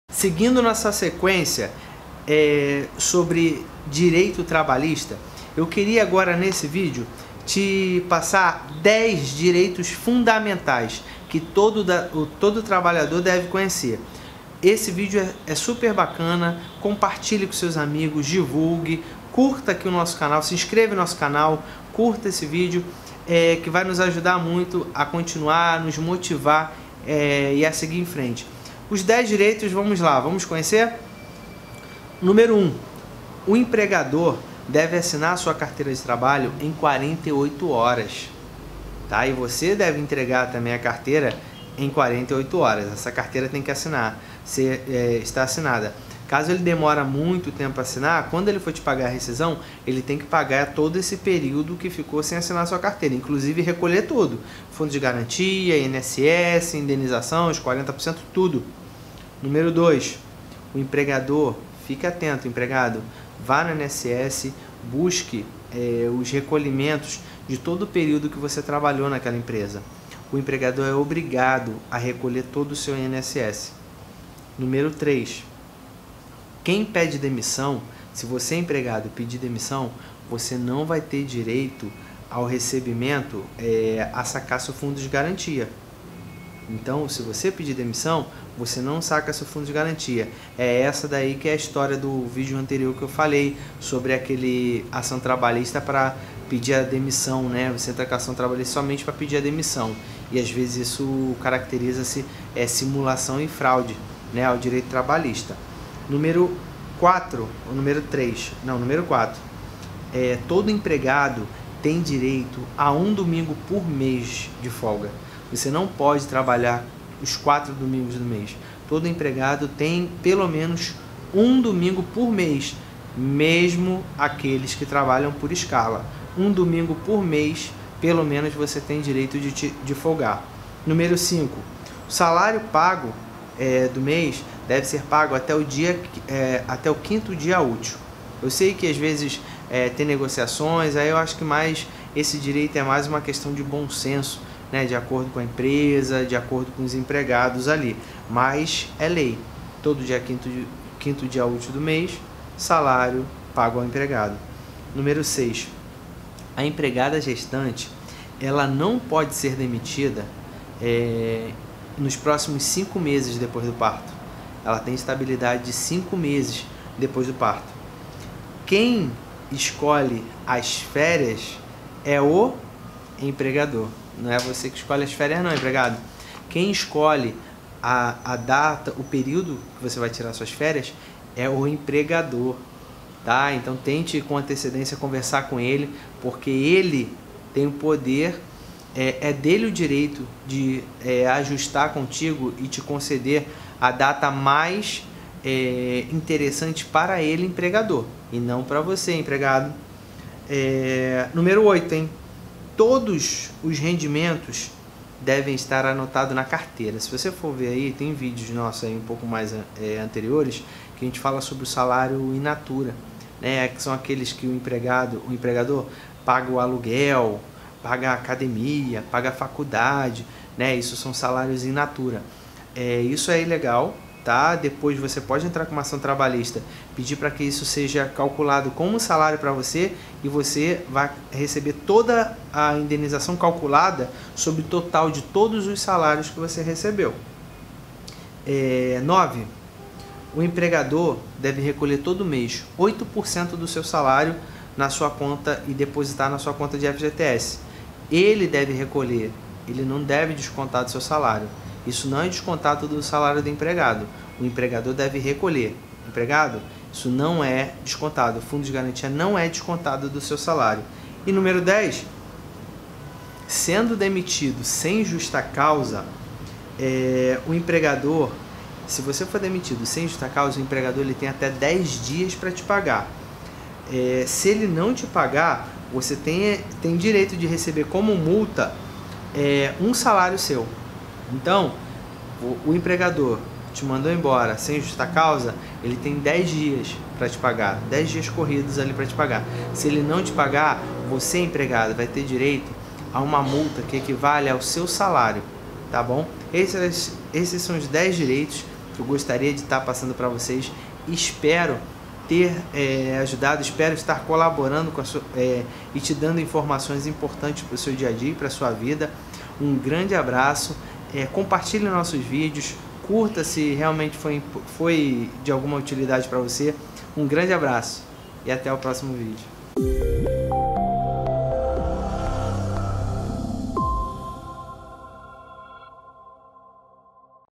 Meus Principais Direitos Trabalhistas. Todo trabalhador deve conhecer.
Como Vencer o Desemprego?

Direitos trabalhistas que todos devem saber

1. O empregado deve assinar a sua carteira em 48 hs;

2. O empregador deve recolher o INSS;

3. Quem pede demissão recebe seguro desemprego, mas não saca FGTS;

4. O empregado tem direito pelo menos um domingo por mês;
5. Salário pago por mês deve ser pago todo o 5º dia útil;

6. Empregada gestante não pode ser demitida desde a concepção até 5 meses após o parto;

7. Quem escolhe a data das férias é o empregador e não o empregado;

8. Todos os rendimentos devem estar anotados na carteira, salário por fora é ilegal;

9. O empregador deve recolher 8% do salário de FGTS, isso não pode ser descontado do empregado;

10. Sendo demitido sem justa causa, o empregador tem 10 dias para pagar toda a sua rescisão trabalhista, senão receber, tem direito a uma multa no valor do seu salário.